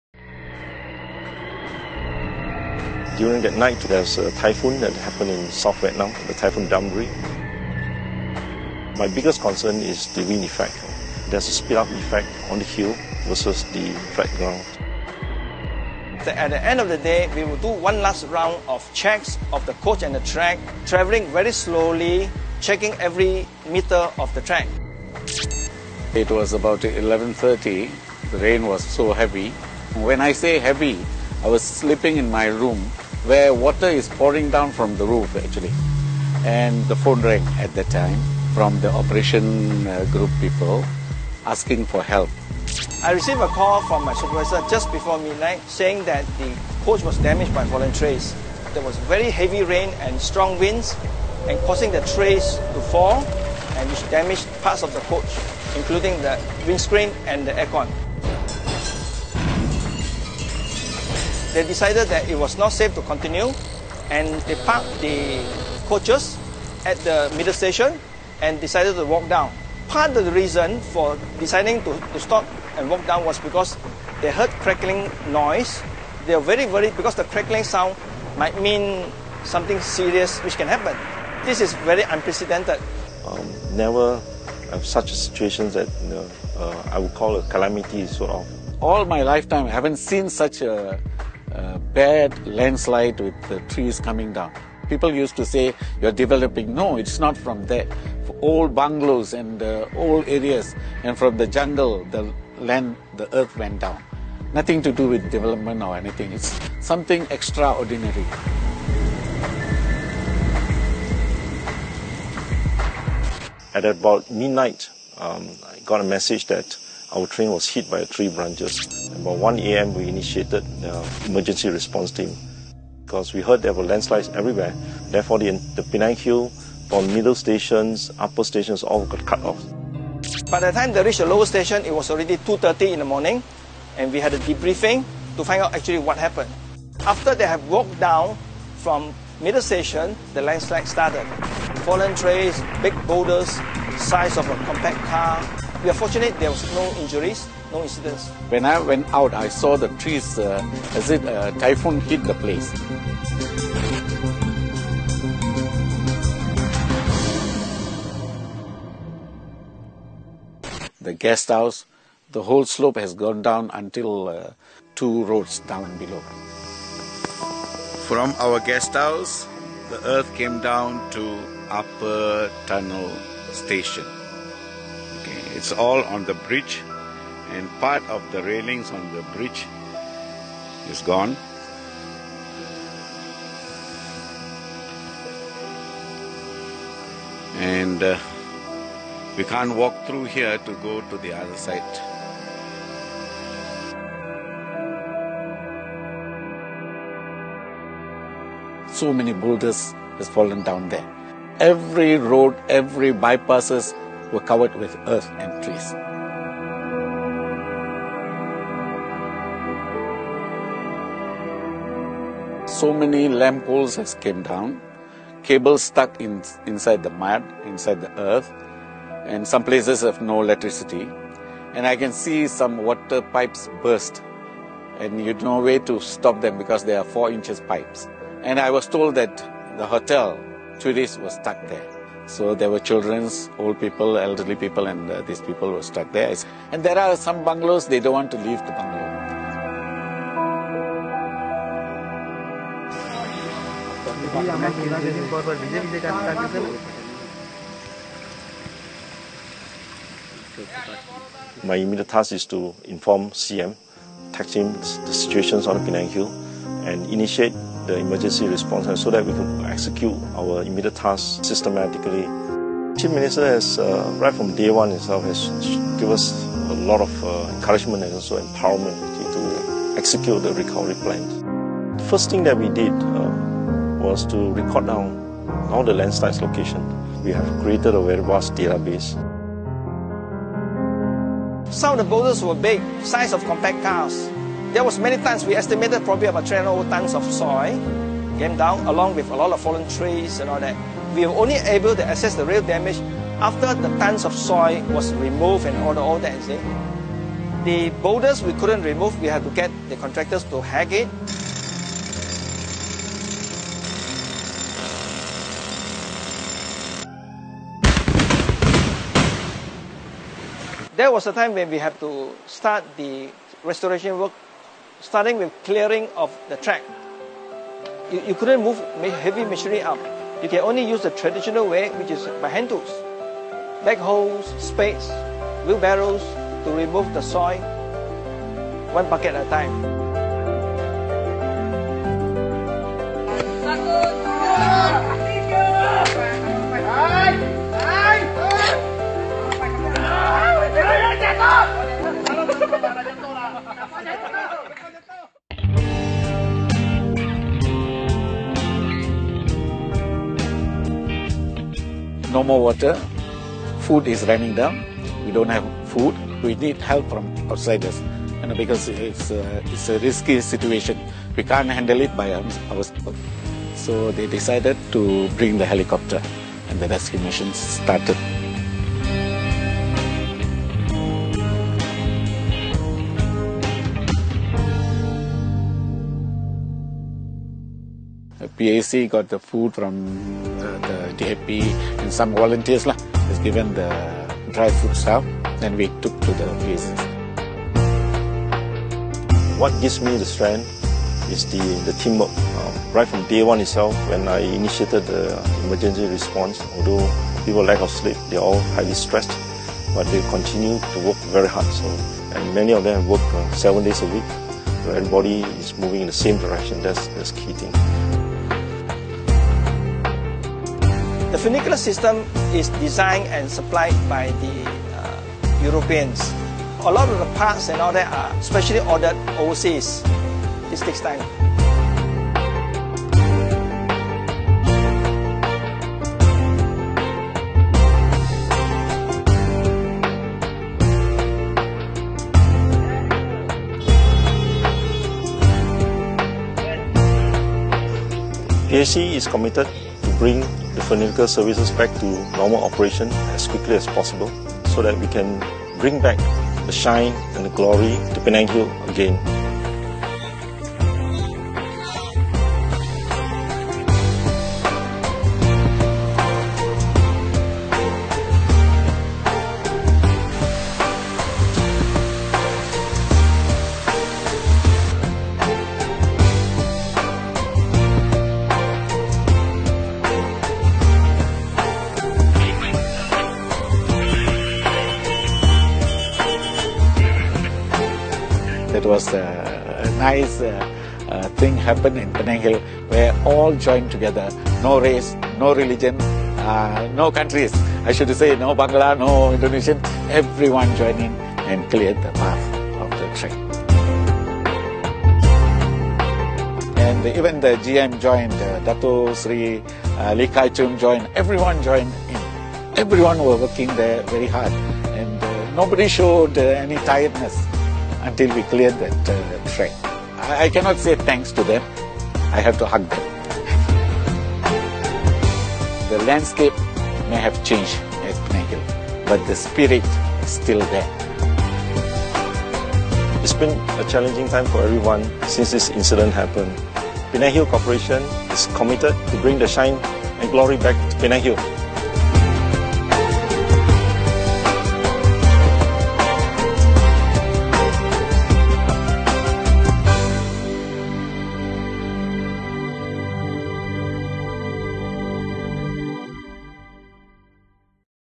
The Storm That Hit Penang Hill
On 4 November 2017, Penang was unfortunate to be hit not just by the Damrey typhoon that hit Vietnam but unexpected fierce 40mph winds that had never seen before in Malaysia.

The extreme weather conditions has caused many landslides, rockfalls, fallen trees and branches along the railway track, jeep track and also on the summit area.

Thanks to the many heroes behind the scenes who have helped in recovering Penang Hill day and night for more than 30 days. Penang Hill was reopened on 30 December 2017.

This short documentary tells the story.